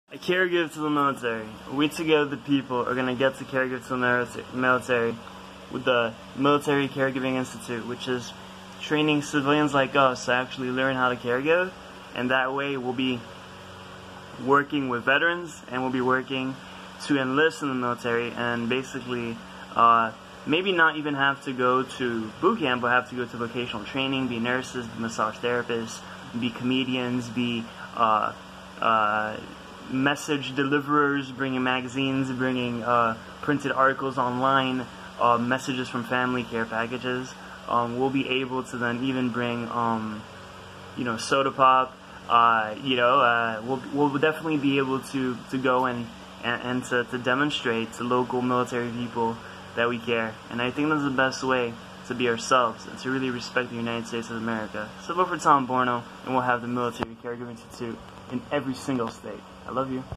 The Military Care Giving Institute, During My Presidency.
We Will Enlist Great Civilians Without Basic Training To Learn The Ethics Of Care Giving And Train Others To Do So In The Military Care Giving Institute. From Comedy, To Massage, Nursing, And Friendship, Conversational Skills, Multi Lingual Teaching Ability As A Core Reward, To Encounter American Heroes, And Maybe Be Too The Same. Vote Now.